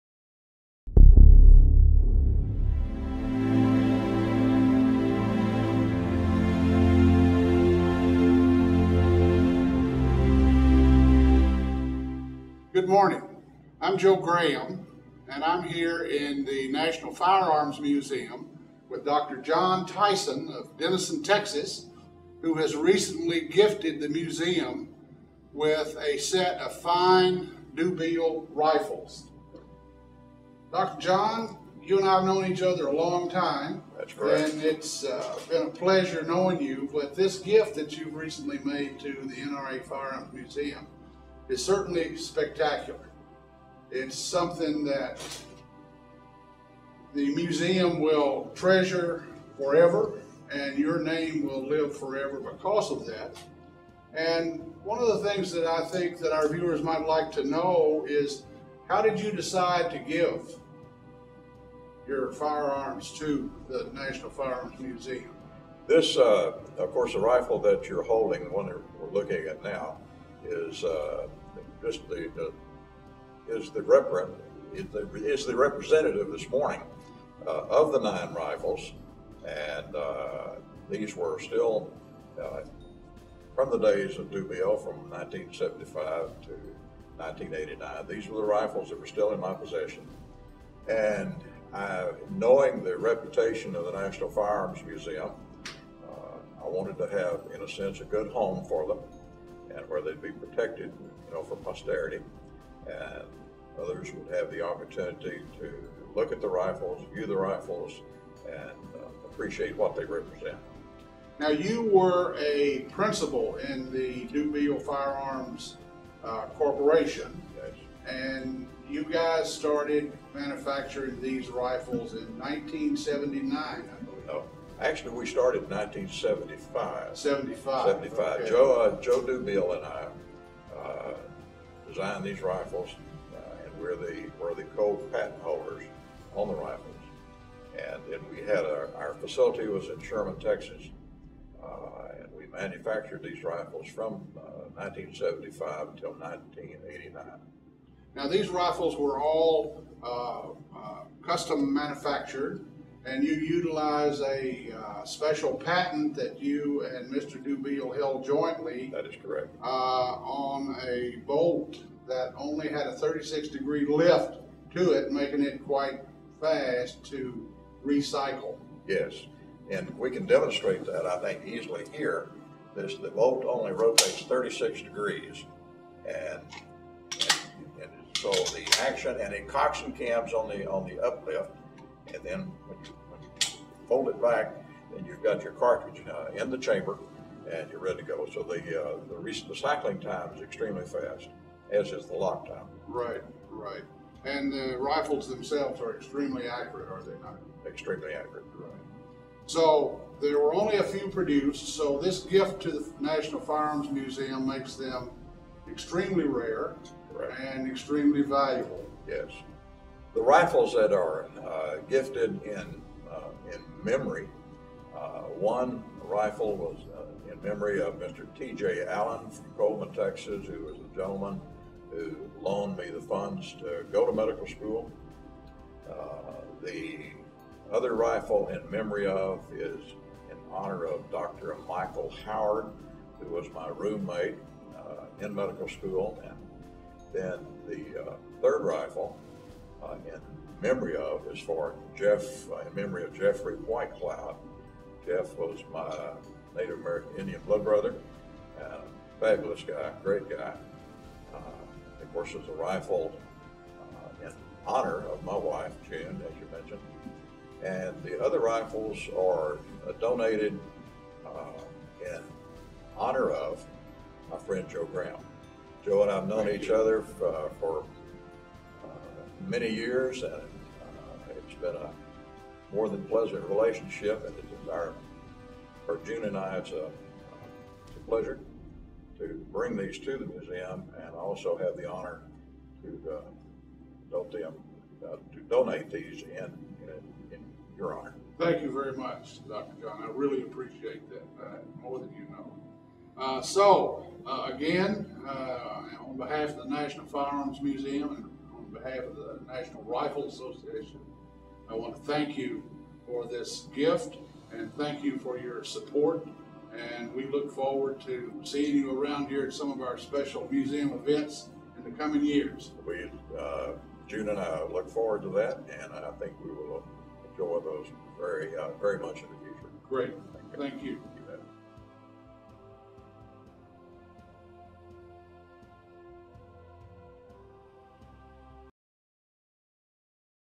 NRA National Firearms Museum DuBiel Rifles Donation
The NRA's Joe Graham talks with Dr. John Tyson, who graciously gifted the National Firearms Museum with a collection of DuBiel Rifles.